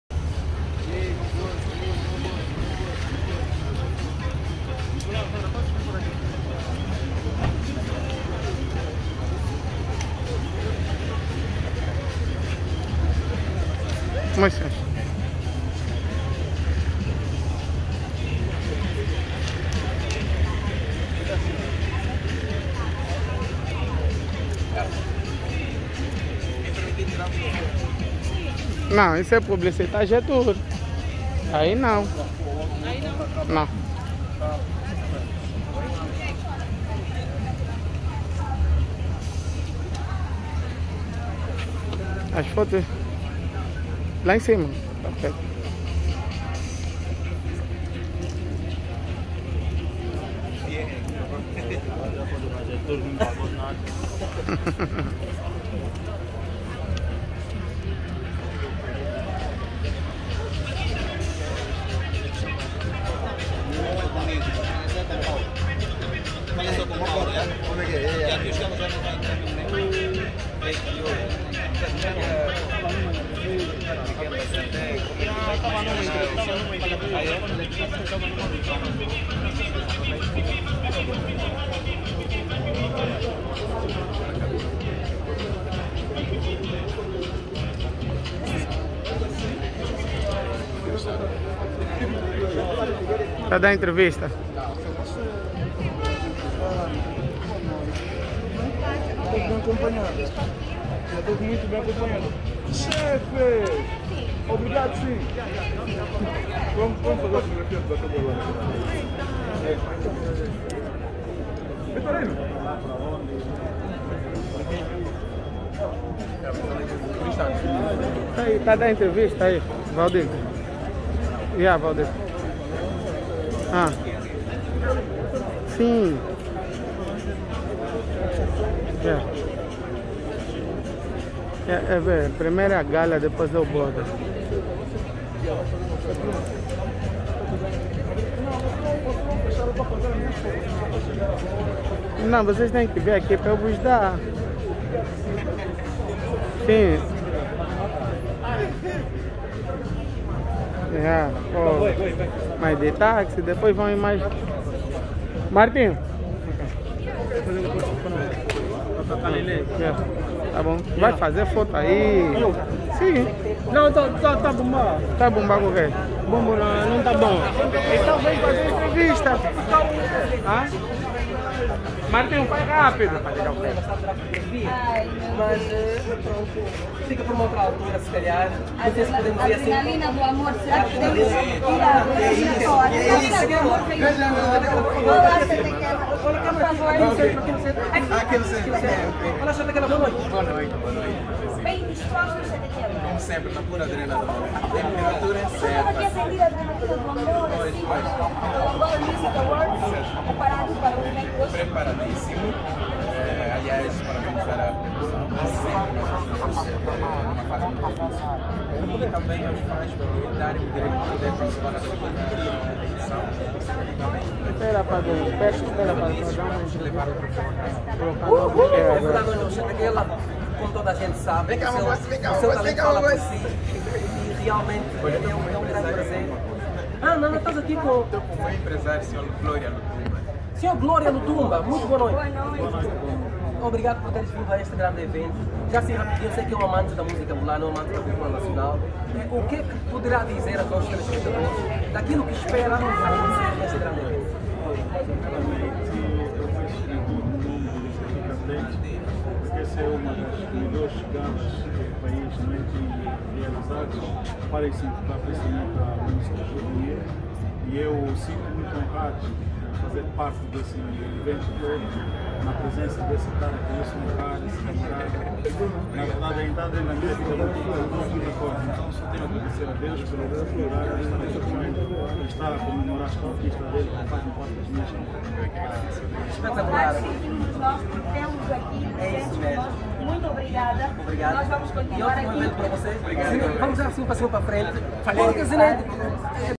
Chetekela - Na chegada no Angola Music Awards com Glória Lutumba.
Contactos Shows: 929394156